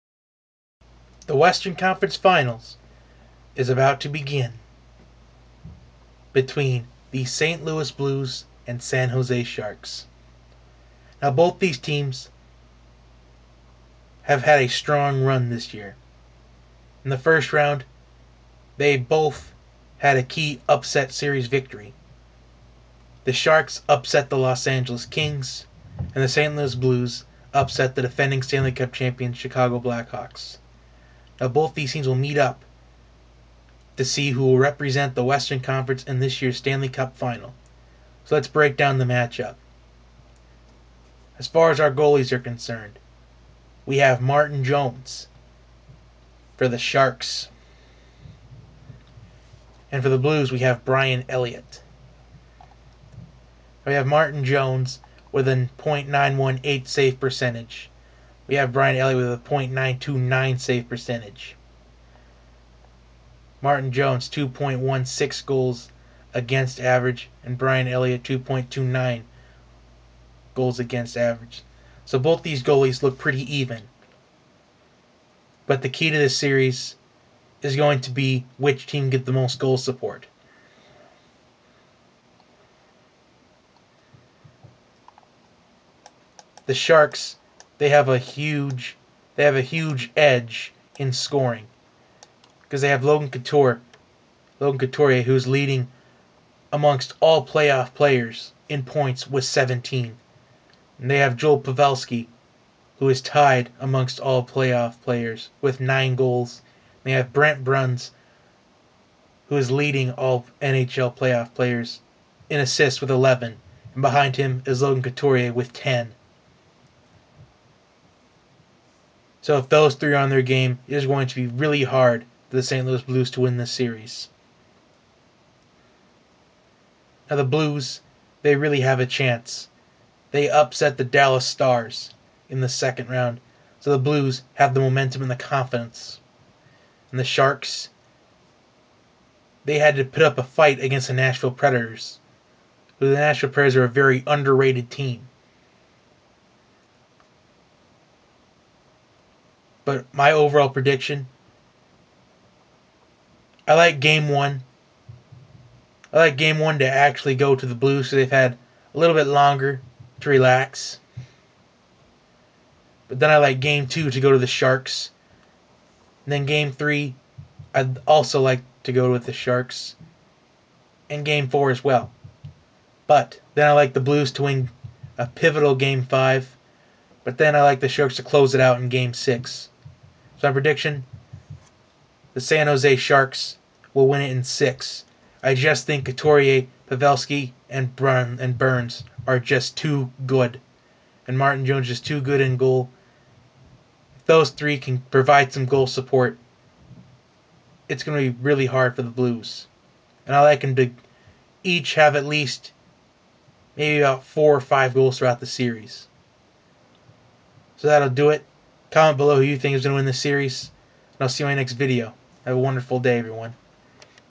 Stanley Cup Playoffs Preview: St.Louis Blues vs. San Jose Sharks
Today I preview the Western Conference Final Matchup between the St.Louis Blues who have already upset the defending cup champions the Chicago Blackhawks and one of the favorites to win it all this year in the Dallas Stars, and the San Jose Sharks who have upset the Los Angeles Kings and coming off a 7 game series against an underrated Nashville Predators team.